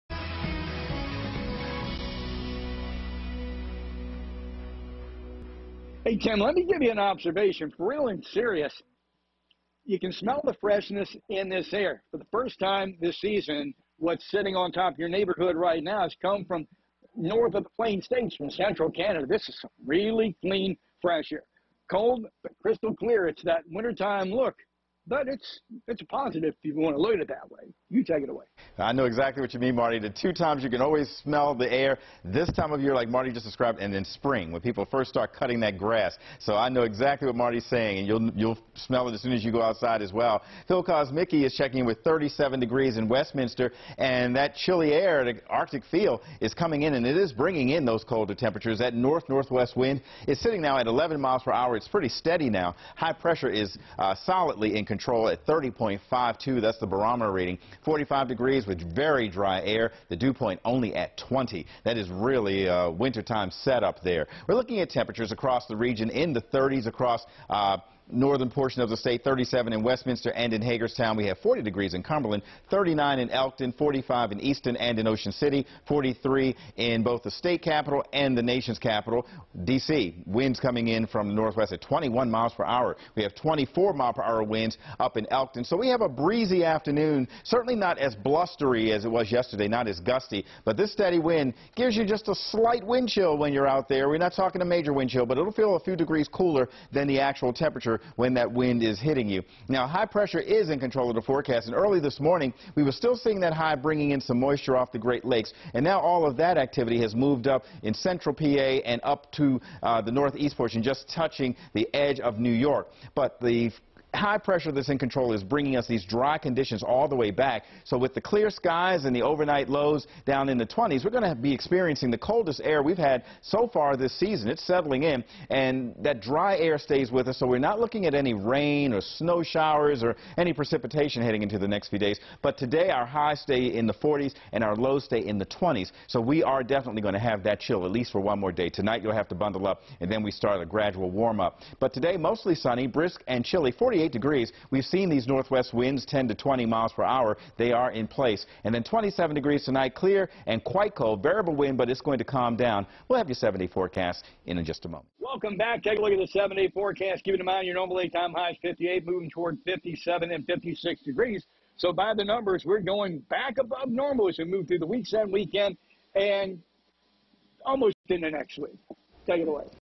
Tim Williams And Marty Bass Have A Look At Your Wednesday Afternoon Forecast
After a chilly Wednesday, temperatures will be back above normal for the weekend.